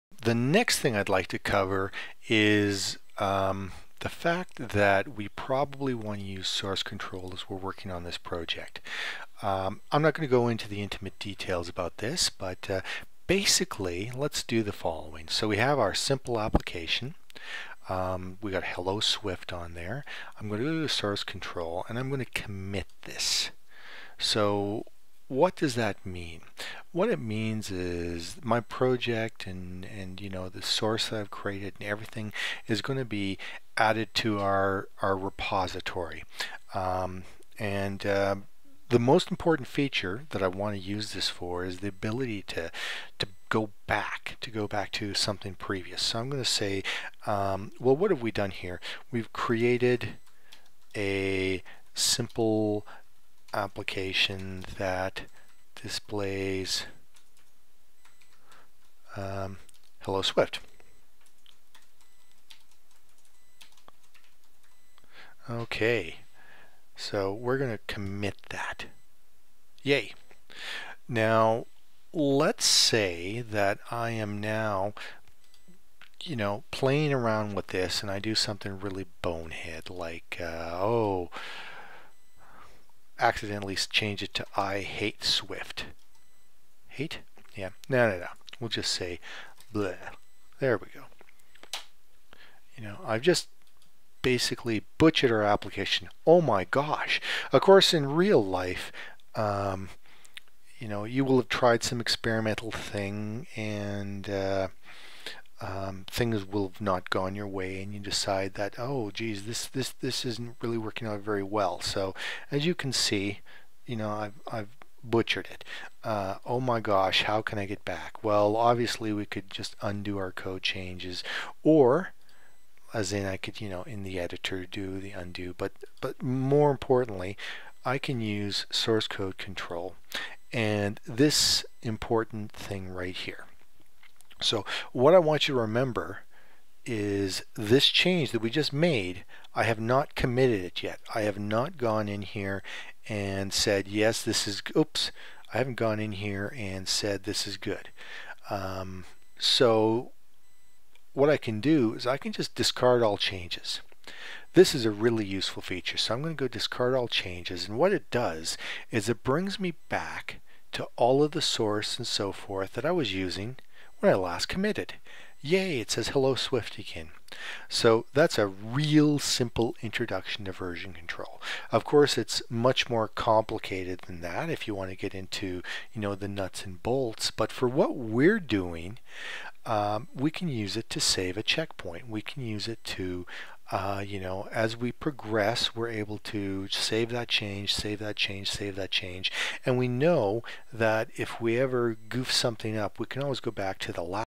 Swift App Dev. 02 - Using built-in Source Control.
In this step we'll review how easy and useful it is to commit your changes in source code control.  This is using "Git" under the hood, and XCode make the simple operations trivial to do.

In this series we're building a simple IOS image manipulation application from the ground up using Swift and XCode.  Simple and Fun!